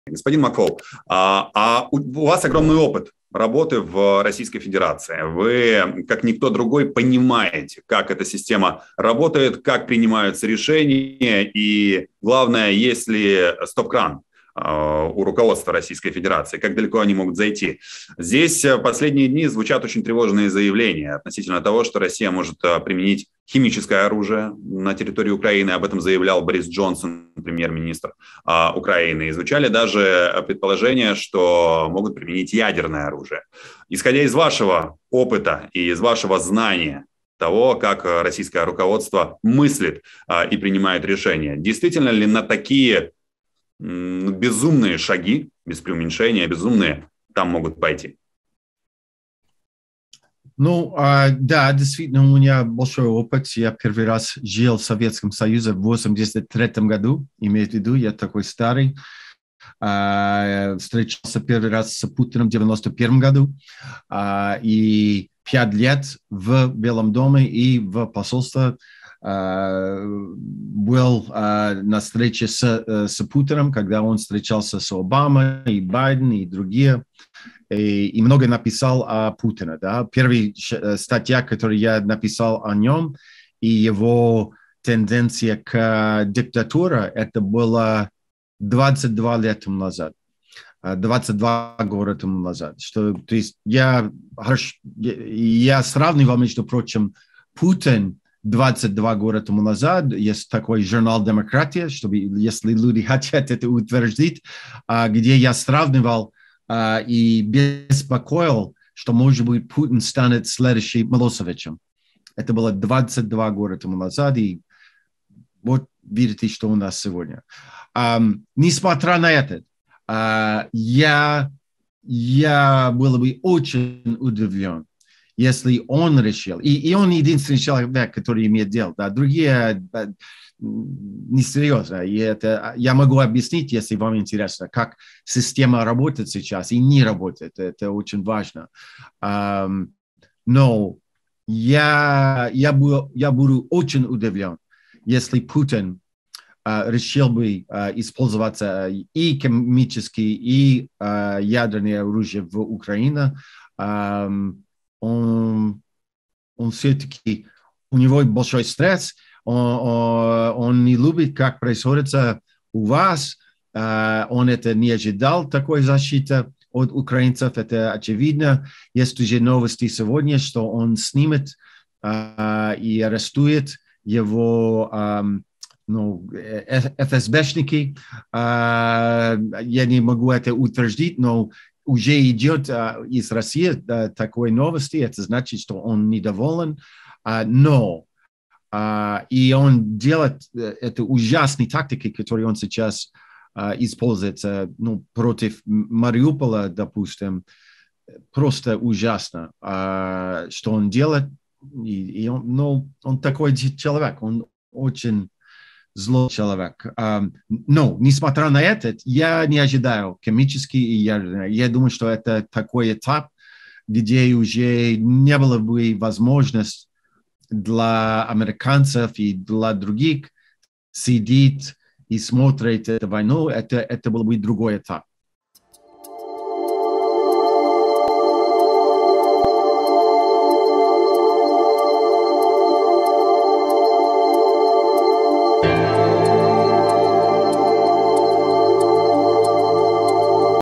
Макфол: Будуть арешти – Путін незадоволений війною / Байден, США, Путін, Росія / Україна 24
"Перша стаття, яку я написав про Путіна та його тенденцію до диктатури, вийшла ще 22 роки тому. Путін зараз не очікував такого результату в Україні і буде арештовувати відповідальних осіб. Те, що він робить з Маріуполем - це дуже жахлива тактика, але, я не очікую і буду дуже здивований, якщо Путін вирішить використати хімічну та ядерну зброю в Україні. Це буде новий етап, за яким США вже не зможуть просто спостерігати", - пояснив американський дипломат, експосол США в Москві Майкл Макфол в ексклюзивному інтерв'ю Володимиру Полуєву на "Україна 24".